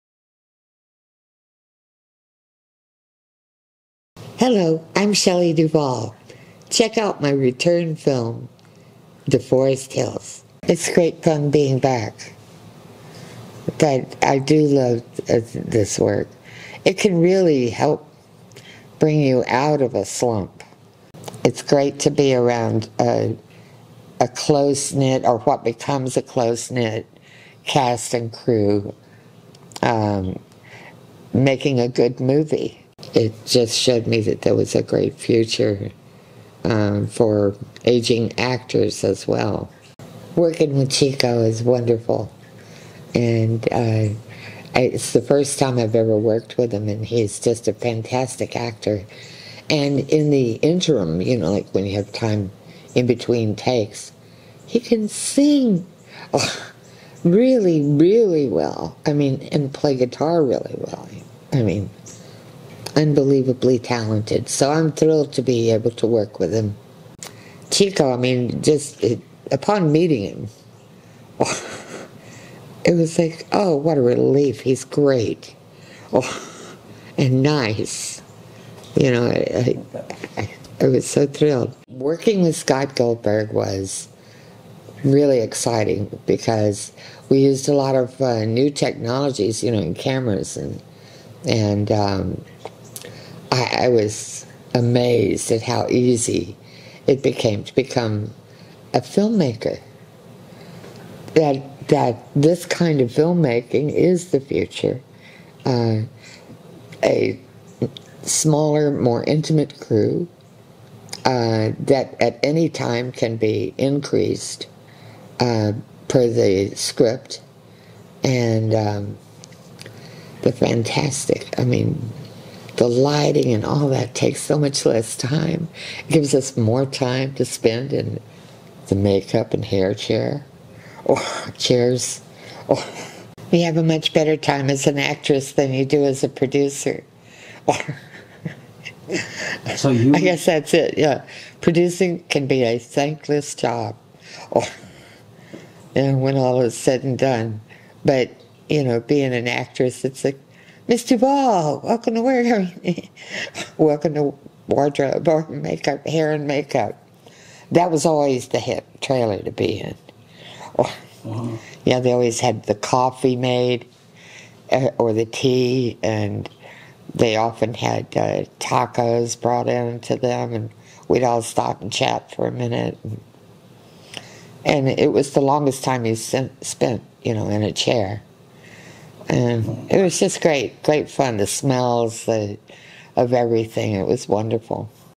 Shelley Duvall Interview (2024) - The Forest Hills
Shelley Duvall talks about various aspects of The Forest Hills.

The Forest Hills will be released this October theatrically, digitally, and on physical media.